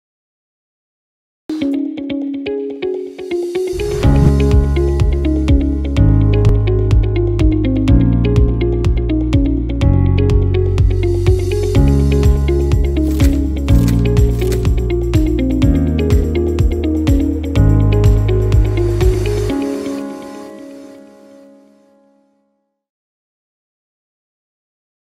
It's time to protect yourself and your family | Tamil
The pandemic has ushered the need for strong immunity. Many Indians have taken the necessary step to safeguard themselves and their loved ones. Check your financial immunity score to know how prepared you are for uncertainties.